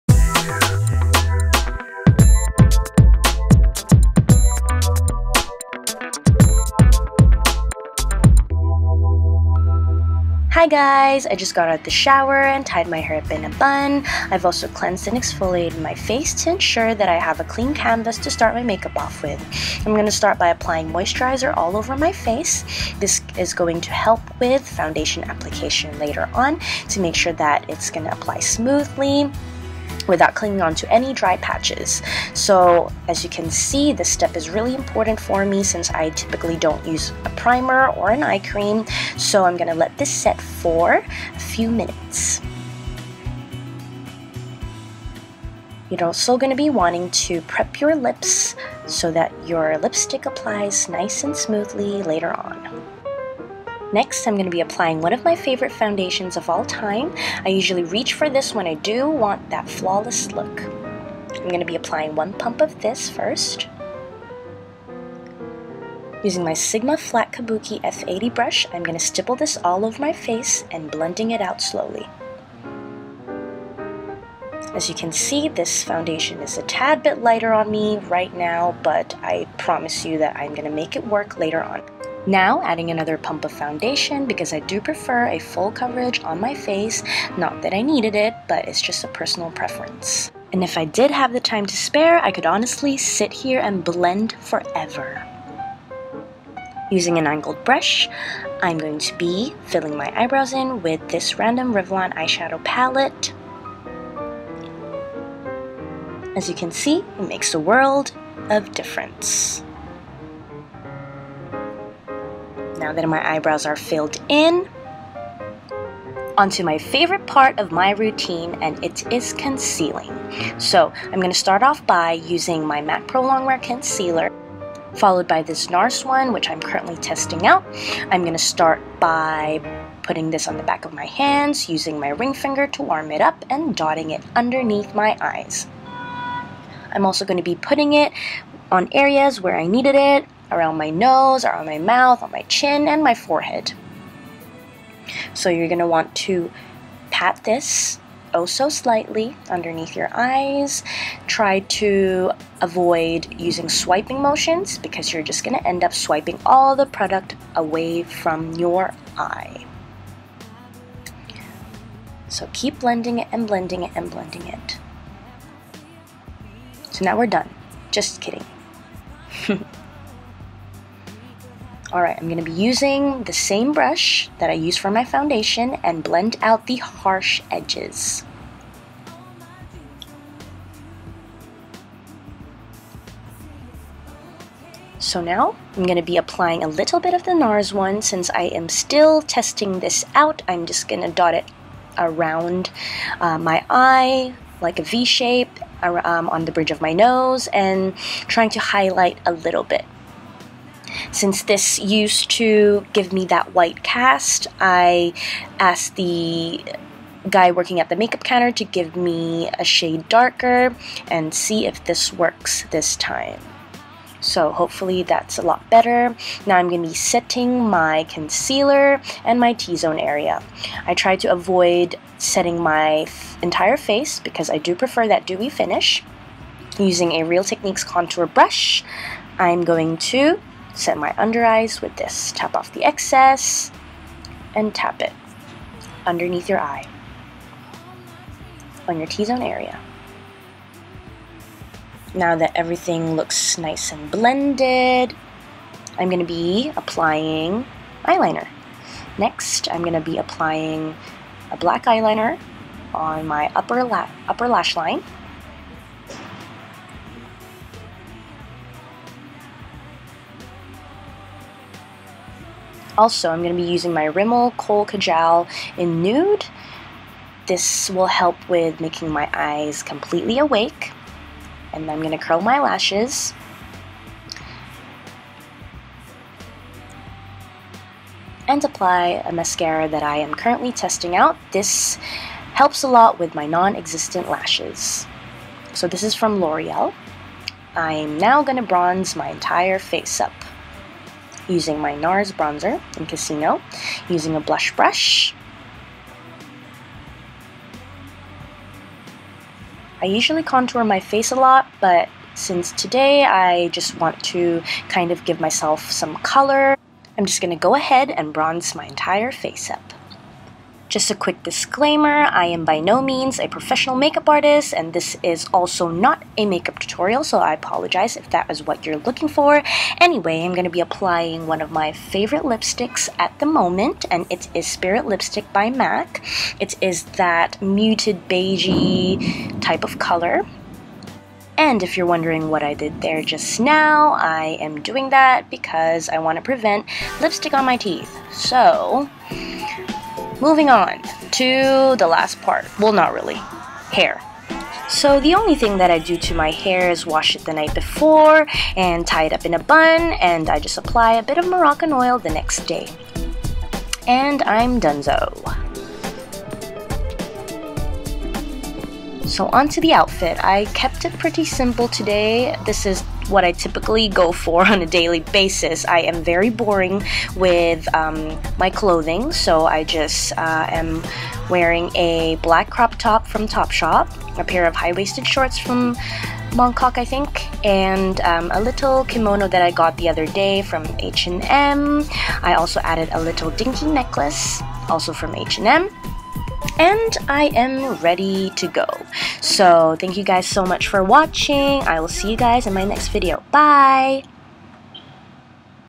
Get Ready With Me: A Casual Weekend | Melizza Villamil
Hey everyone! [A Quick Disclaimer] I am by no means a professional makeup artist, this is not meant to be a makeup tutorial whatsoever. This video mainly focuses on what I do on a daily basis.

Melizza

Software I Used To Edit:
- iMovie